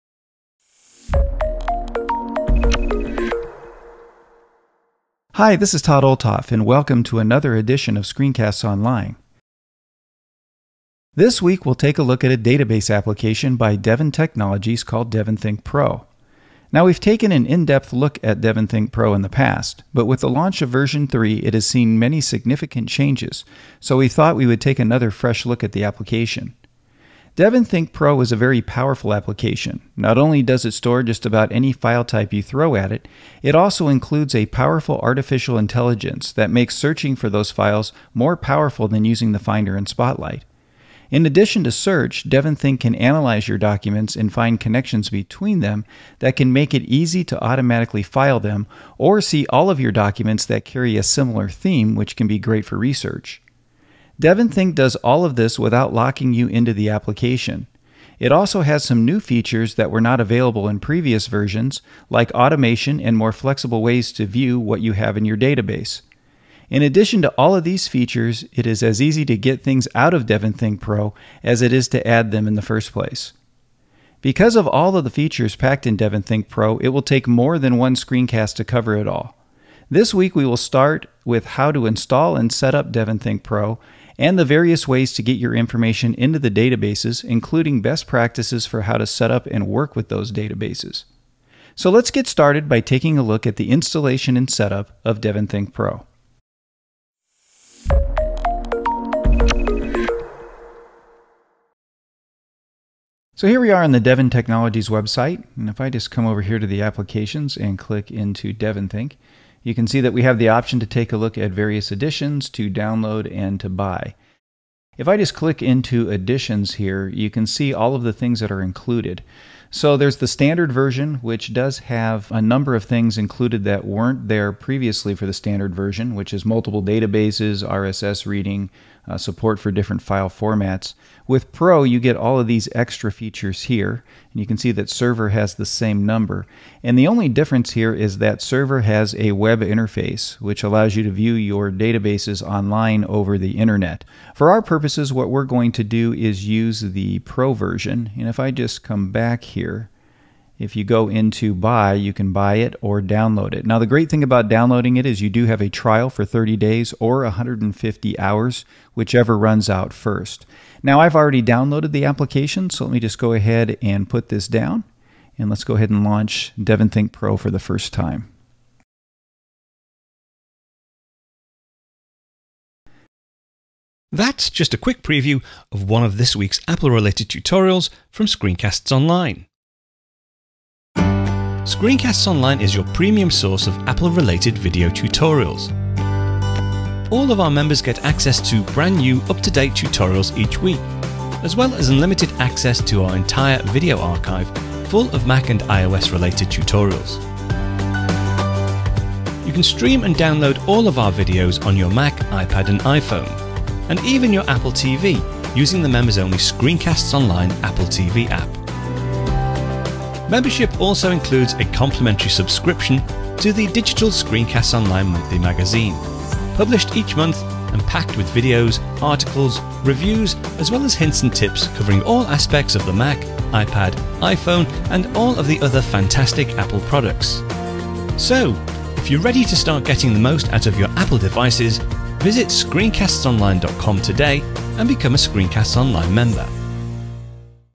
SCOM0893 - DEVONthink 3 Foundation - Preview
This is a short preview of a full ScreenCastsOnline video tutorial. for a free 7-day membership. - DEVONthink is one of the most powerful database applications made for the Mac. In fact, some people have said this one application makes it worth getting a Mac just to run it!

In this first of three foundation screencasts, Todd covers how to install and set up DEVONThink for the first time. He then walks you through all of the ways to get information into your databases. DEVONThink is a really rich application that has a plug-in for just about every type of information gathering you can think of, and some you may not have thought of!